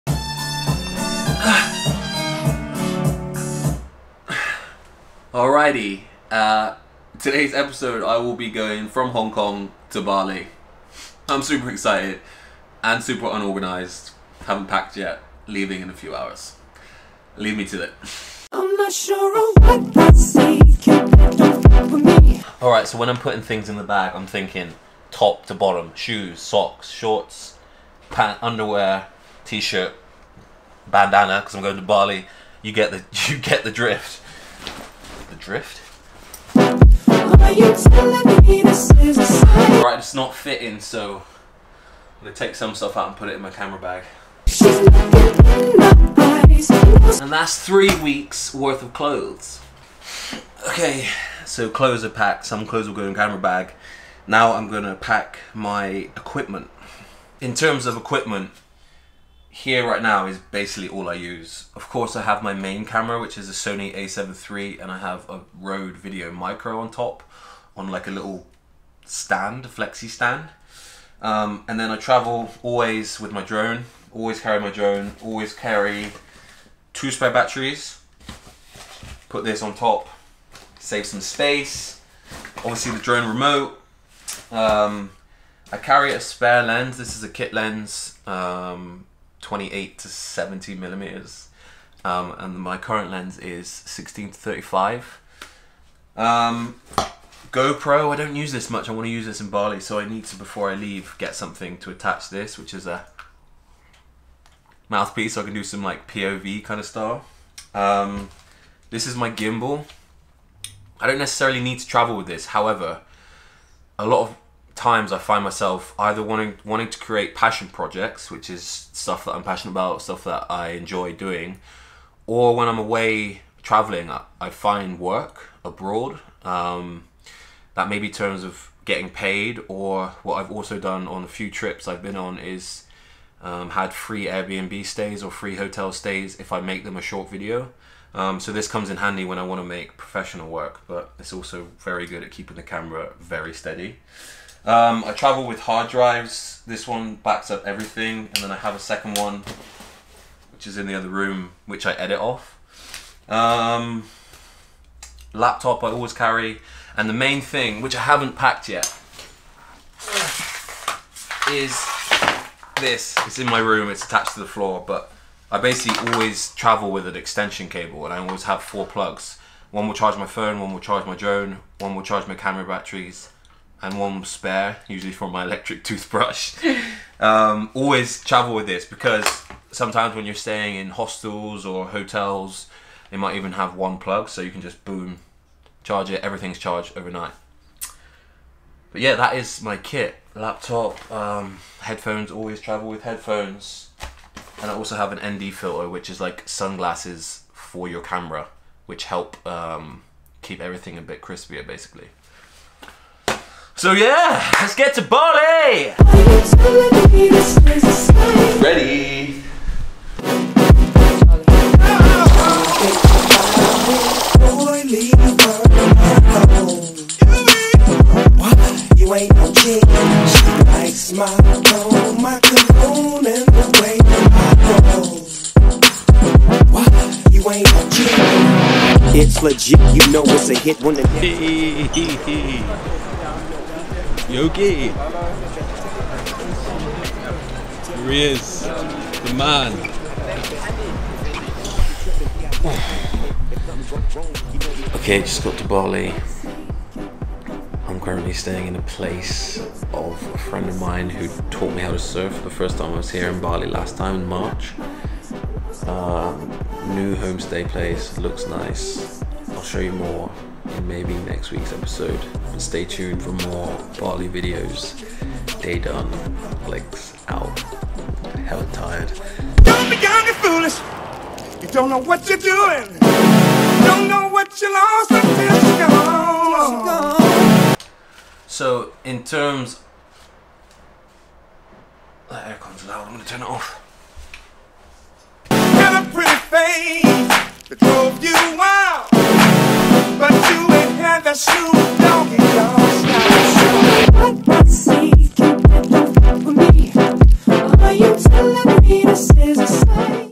What I pack when I travel to Bali - Ep.13
Heading to Bali for 3 weeks. Here's what i pack when i travel.

FOLLOW ME

SONGS IN THE VLOG——